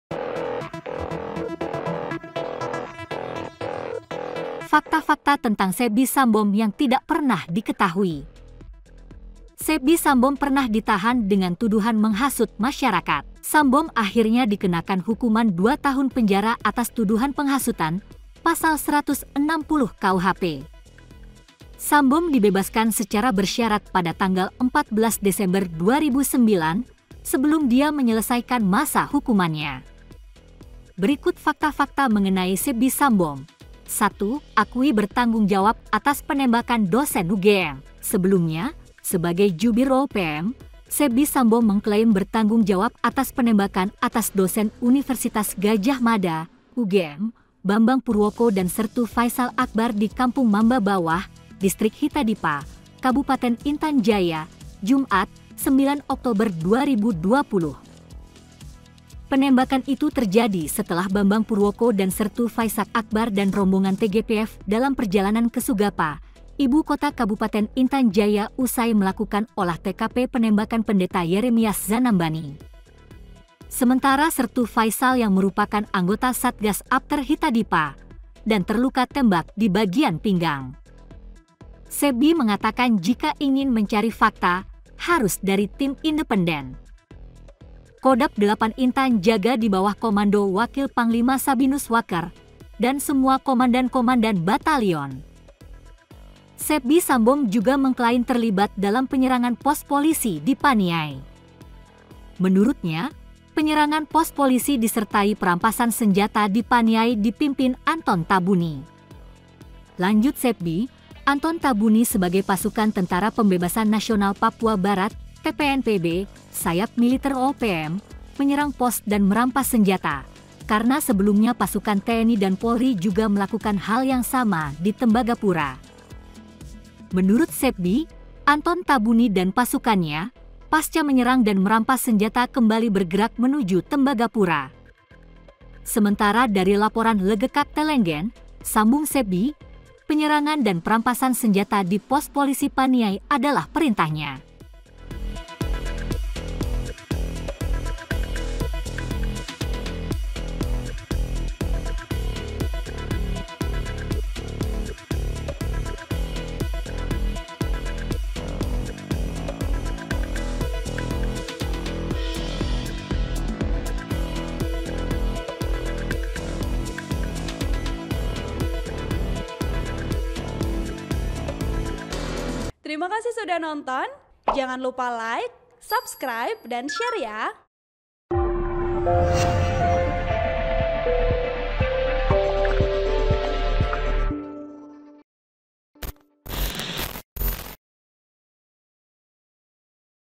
Fakta-Fakta Tentang Sebby Sambom yang Tidak Pernah Diketahui
POS-KUPANG.COM - Sebby Sambom Pernah ditahan dengan Tuduhan Menghasut Masyarakat

Sambom akhirnya dikenakan hukuman dua tahun penjara atas tuduhan penghasutan (Pasal 160 KUHP).

Sambom dibebaskan secara bersyarat pada tanggal 14 Desember 2009, sebelum dia menyelesaikan masa hukumannya.
(POS-KUPANG.COM, Putra Dewangga Candra Seta)

Artikel ini telah tayang di Surya.co.id dengan judul Biodata Sebby Sambom Tokoh KKB Papua yang Sok-sokan Tanggapi Penangkapan Lukas Enembe: Jubir OPM,

Penulis : Putra Dewangga Candra Seta
Editor : Musahadah
VPO : Lenny